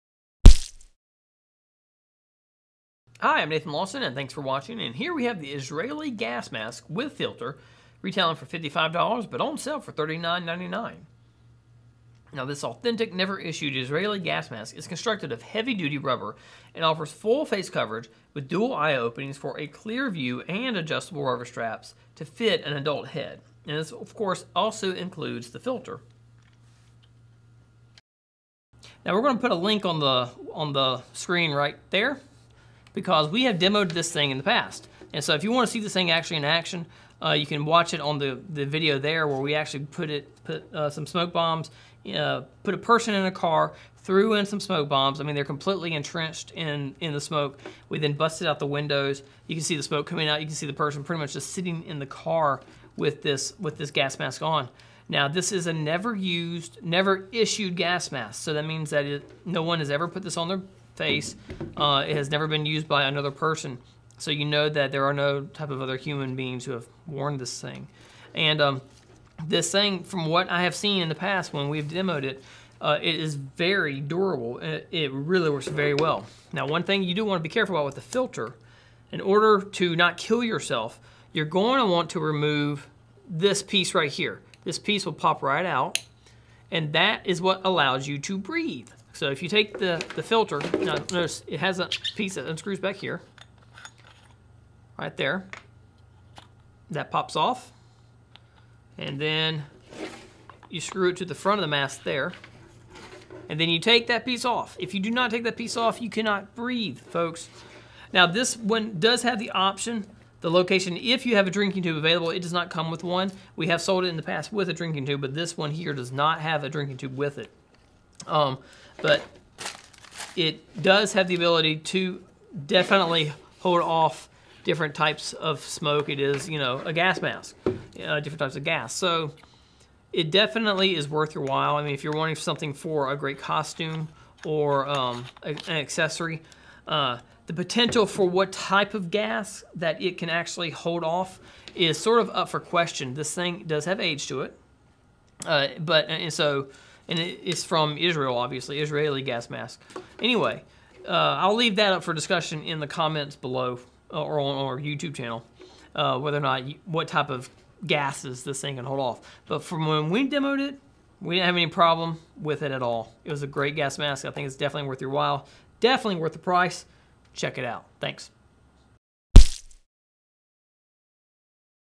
Israeli Gas Mask With Filter
This authentic, never-issued Israeli gas mask is constructed of heavy-duty rubber and offers full-face coverage with dual eye-openings for a clear view and adjustable rubber straps to fit an adult head. Includes filter.